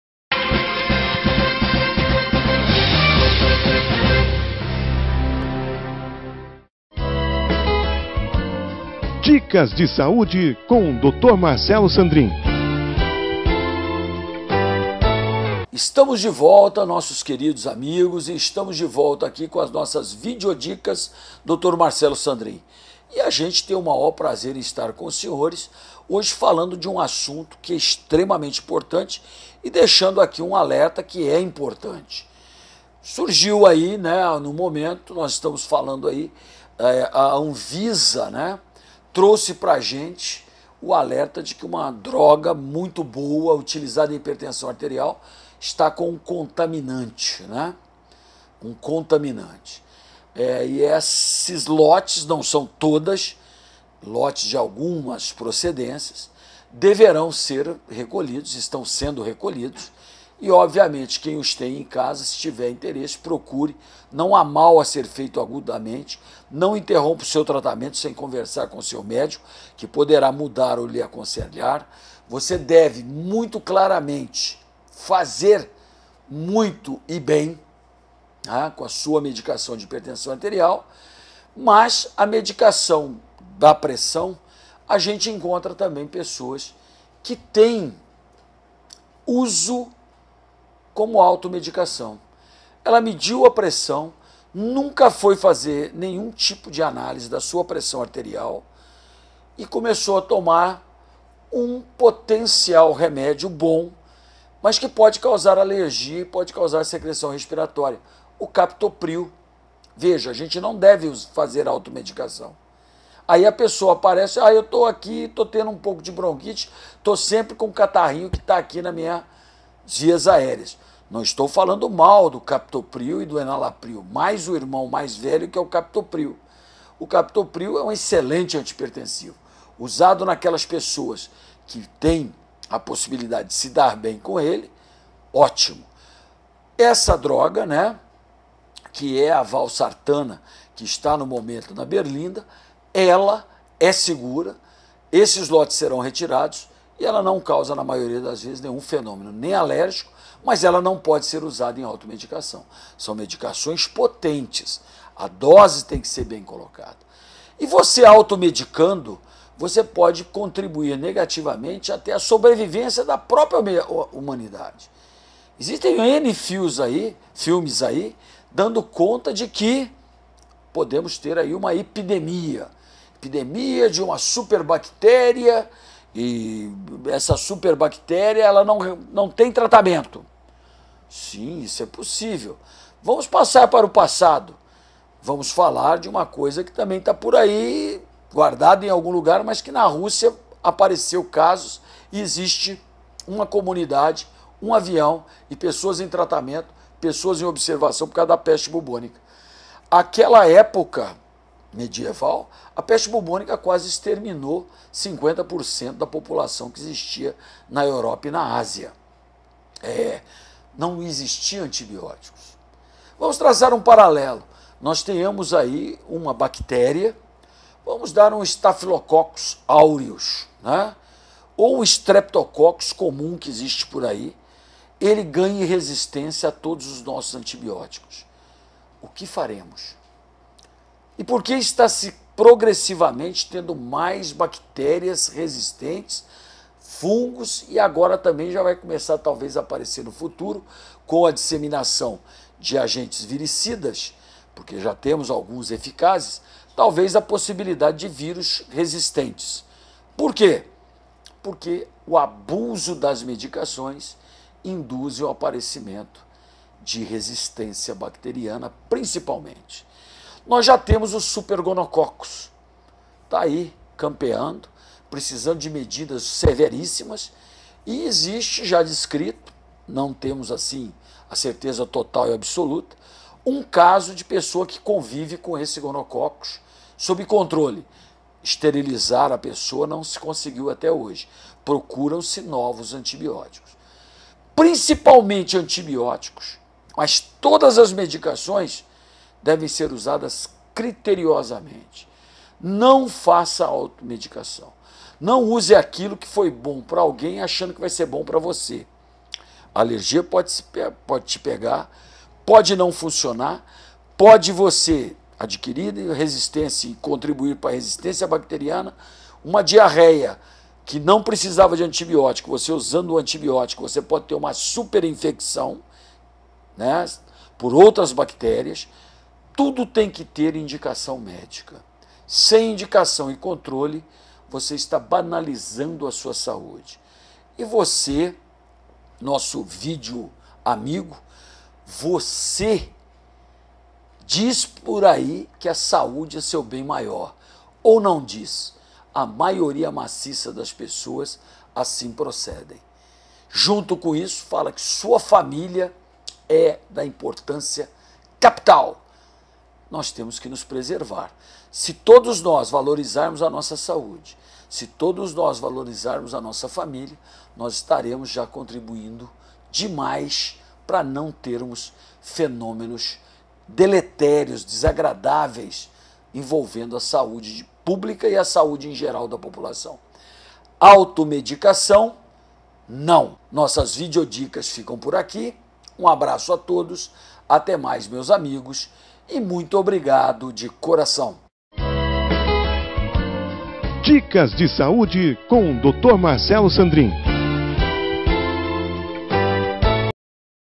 Dr Marcelo Sandri - Cuidado com a Automedicação
A automedicação se caracteriza pela prática de tomar remédios, sem a avaliação prévia de um profissional de saúde.